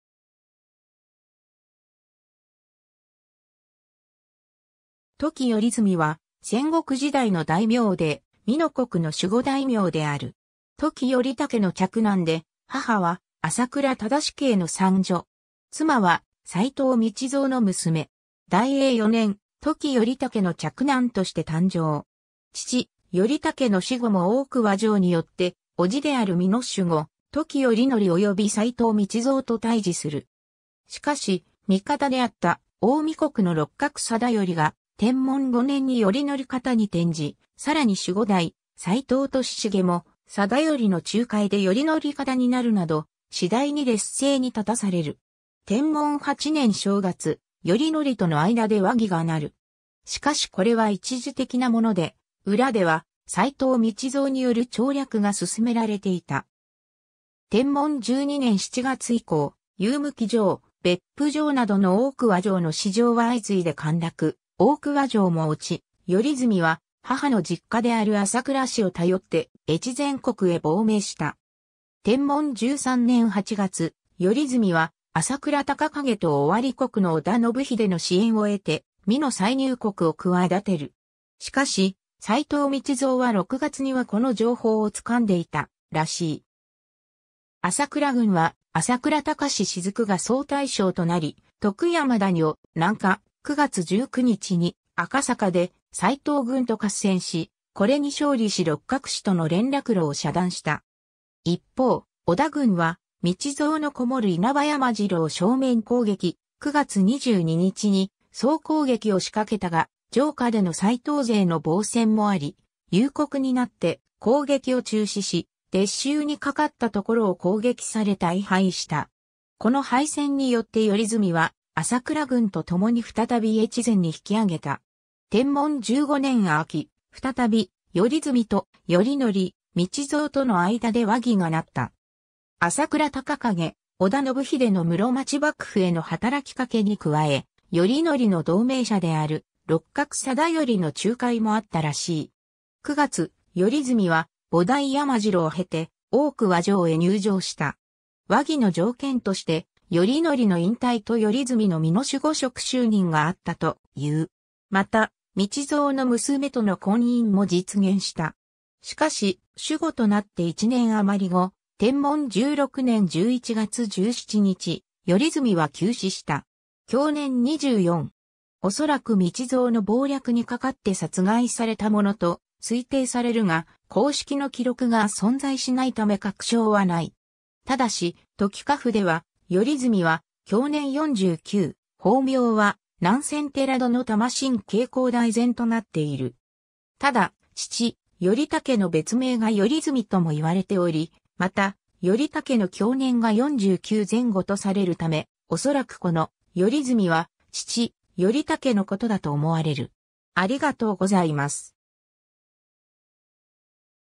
土岐頼純
土岐 頼純（とき よりずみ）は、戦国時代の大名で美濃国の守護大名である。
土岐頼武の嫡男で、母は朝倉貞景の三女。
妻は斎藤道三の娘。
 大永4年（1524年）、土岐頼武の嫡男として誕生。
 父・頼武の死後も大桑城に拠って、叔父である美濃守護・土岐頼芸および斎藤道三と対峙する。
しかし、味方であった近江国の六角定頼が天文5年（1536年）に頼芸方に転じ、さらに守護代・斎藤利茂も定頼の仲介で頼芸方になるなど、次第に劣勢に立たされる。
 天文8年（1539年）正月、頼芸との間で和議が成る。
しかしこれは一時的なもので、裏では斎藤道三による調略が進められていた。
天文12年（1543年）7月以降、祐向城、別府城などの大桑城の支城は相次いで陥落、大桑城も落ち、頼純は母の実家である朝倉氏を頼って越前国へ亡命した。
 天文13年（1544年）8月、頼純は朝倉孝景（宗淳）と尾張国の織田信秀の支援を得て、美濃再入国を企てる。
しかし、斎藤道三は6月にはこの情報をつかんでいたらしい。
朝倉軍は朝倉宗滴が総大将となり、徳山谷を南下、9月19日に赤坂で斎藤軍と合戦し、これに勝利し六角氏との連絡路を遮断した。
一方、織田軍は道三の籠もる稲葉山城を正面攻撃、9月22日に総攻撃を仕掛けたが、城下での斎藤勢の防戦もあり、夕刻になって攻撃を中止し、撤収にかかったところを攻撃され大敗した。
この敗戦によって頼純は朝倉軍とともに再び越前に引き揚げた。
 天文15年（1546年）秋、再び頼純と頼芸・道三との間で和議が成った。
朝倉孝景、織田信秀の室町幕府への働きかけに加え、頼芸の同盟者である六角定頼の仲介もあったらしい。
9月、頼純は菩提山城を経て大桑城へ入城した。
和議の条件として、頼芸の隠退と頼純の美濃守護職就任があったという。
また、道三の娘との婚姻も実現した。
 しかし、守護となって1年余り後、天文16年（1547年）11月17日、頼純は急死した。
享年24。
おそらく道三の謀略にかかって殺害されたものと推定されるが、公式の記録が存在しないため確証はない。
 ただし、『土岐家譜』では、頼純は享年49、法名は南泉寺殿玉岑珪公大禅となっている。
ただ、父・頼武の別名が頼純とも言われており、また頼武の享年が49前後とされるため、おそらくこの「頼純」は、父・頼武のことだと思われる。